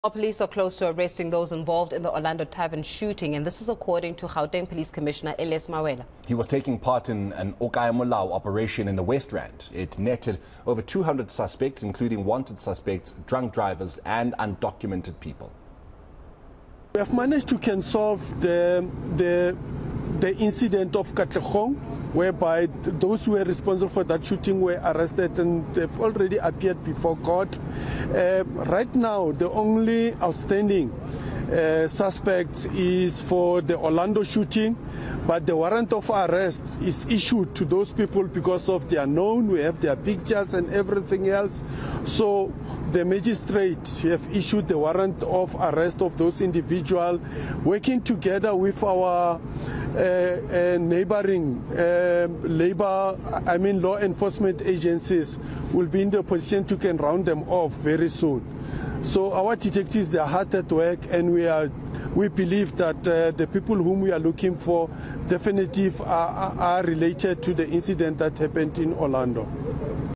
Police still chasing Orlando suspects
Police have once again vowed to bring to book those in the massacre of revellers at a tavern in Orlando.